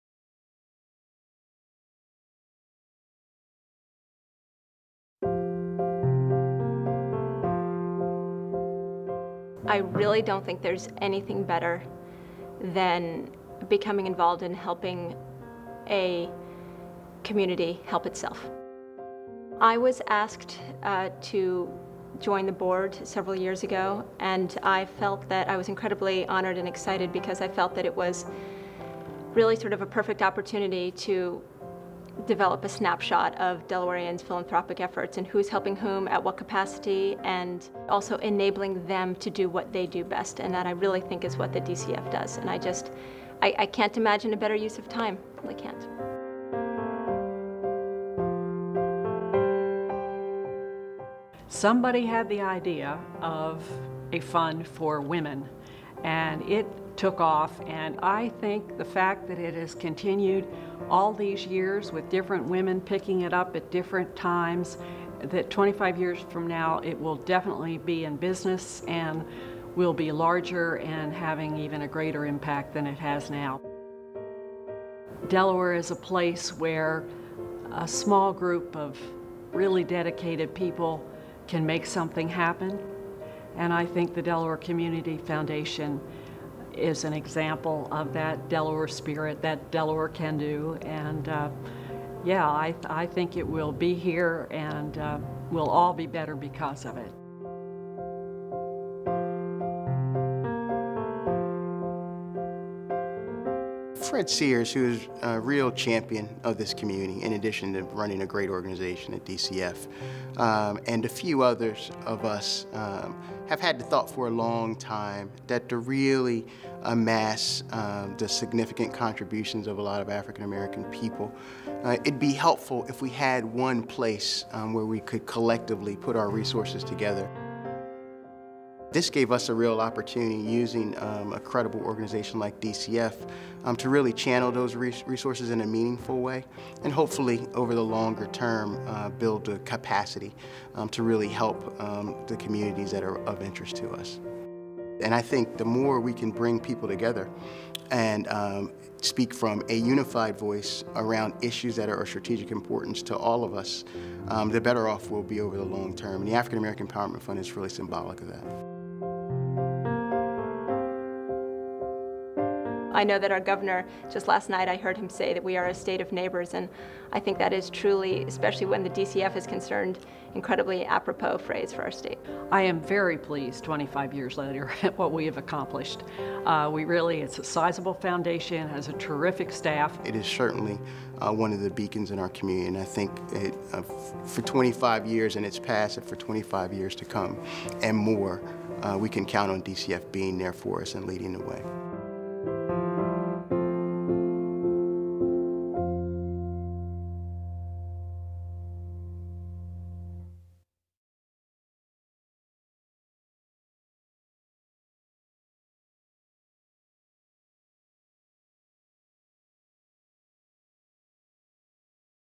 Delaware Community Foundation: Building the Vision
Tony Allen, Judy Hoopes and Laurisa Schutt discuss how the Delaware Community Foundation has gained strength and momentum since its inception in 1986. This video was created as part of the 25th Anniversary Celebration of the Delaware Community Foundation.